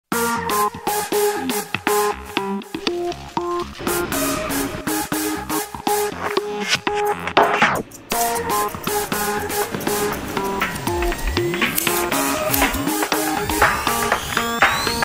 SHIHTZU, GROOMING ✂️❤️🐶
Korean Grooming Techniques!
Total transformation of a shihtzu with  grooming, cuteness guaranteed!

Hello! I'm Bell, a  2 years old beautiful shih tzu!! Today was my grooming with scissor at Tosa Bebe Pet Center. What did you think? Did I get cuter still?? kisses✂️✂️✂️💋💋🐶